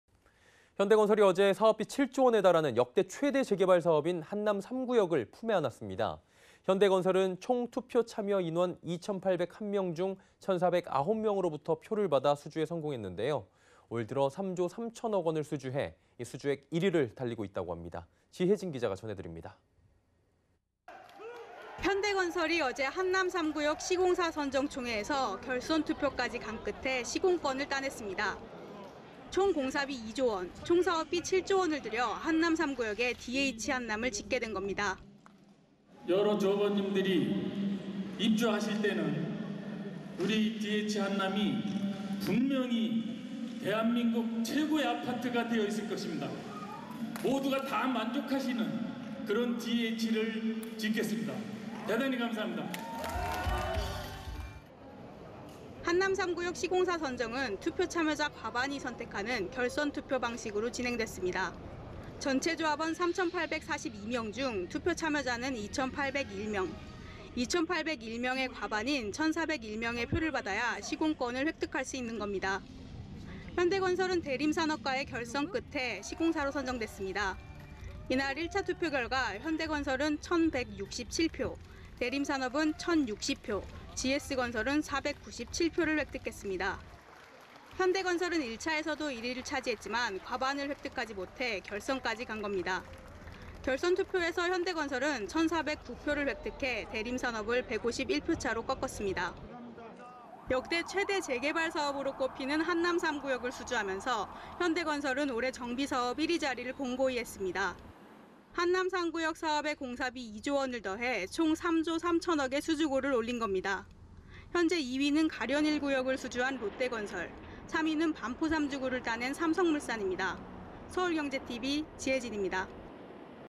한남3구역 품은 현대건설, 올해 3.3조 수주 선두
[앵커]
현대건설이 어제(21일) 사업비 7조원에 달하는 역대 최대 재개발 사업인 한남3구역을 품에 안았습니다. 현대건설은 총 투표참여 인원 2,801명 중 1,409명으로부터 표를 받아 수주에 성공했는데요. 올 들어 3조3,000억원을 수주해 수주액 1위를 달리고 있습니다. 지혜진기자입니다.

[기자]
현대건설이 어제(21일) 한남3구역 시공사 선정 총회에서 결선투표까지 간 끝에 시공권을 따냈습니다.
총공사비 2조원, 총사업비 7조원을 들여 한남3구역에 ‘디에이치 한남’을 짓게 된 겁니다.

[싱크] 윤영준 / 현대건설 주택본부사업장(부사장)
“여러 조합원님들이 입주하실 때에는 우리 디에이치 한남이 분명히 대한민국 최고의 아파트가 되어있을 것입니다. 모두가 다 만족할 수 있는 그런 디에이치를 짓겠습니다. 대단히 감사합니다.”

한남3구역 시공사 선정은 투표 참여자 과반이 선택하는 결선투표 방식으로 진행됐습니다.
전체 조합원 3,842명 중 투표 참여자는 2,801명(사전투표자 66명 현장참여자 2,735명).
2,801명의 과반인 1,401명의 표를 받아야 시공권을 획득할 수 있는 겁니다.

현대건설은 대림산업과의 결선 끝에 시공사로 선정됐습니다.
이날 1차 투표 결과 현대건설은 1,167표, 대림산업 1,060표, GS건설 497표를 획득했습니다.
현대건설은 1차에서도 1위를 차지했지만 과반을 획득하지 못해 결선까지 간 겁니다.
결선투표에서 현대건설은 1,409표를 획득해 대림산업(1,258표)을 151표차로 꺾었습니다.

역대 최대 재개발 사업으로 꼽히는 한남3구역을 수주하면서 현대건설은 올해 정비사업 1위 자리를 공고히 했습니다.
한남3구역 사업의 공사비 2조원을 더해 총 3조3,000억원의 수주고를 올린 겁니다. 현재 2위는 갈현1구역을 수주한 롯데건설(1조5,887억원), 3위는 반포3주구를 따낸 삼성물산(1조487억원)입니다.